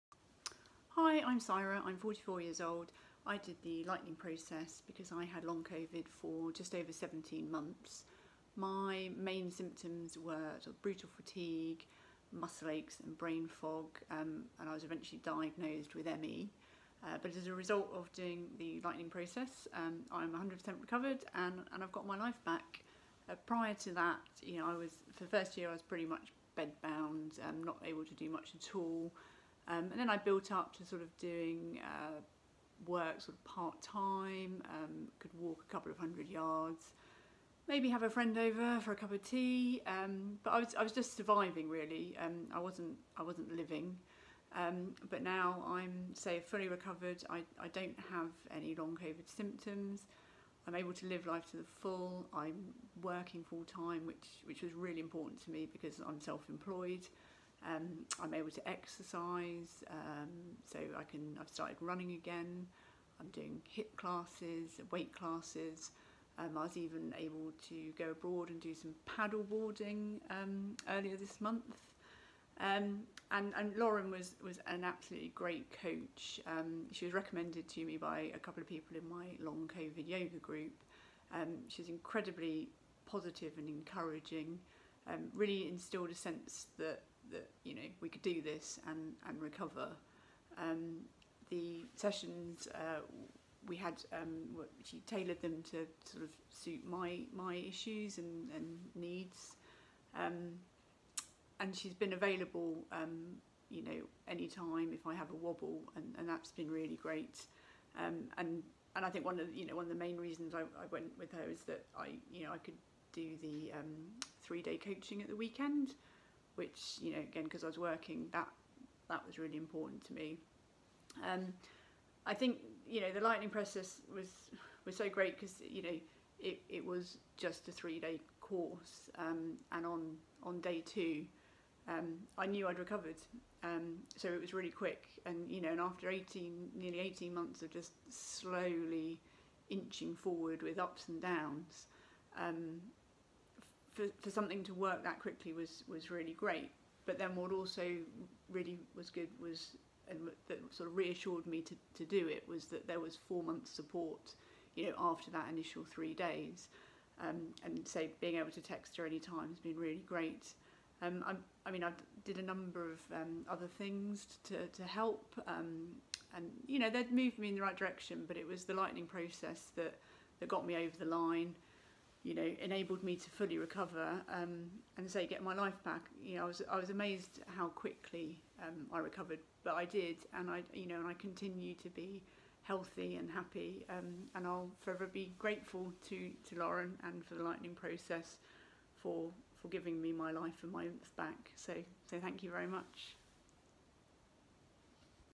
Lightning Process: I got my life back!
In this video, Cyra talks about having Long Covid and ME for 17 months and how she is now back to full health after taking the Lightning Process with Lauren Stoney!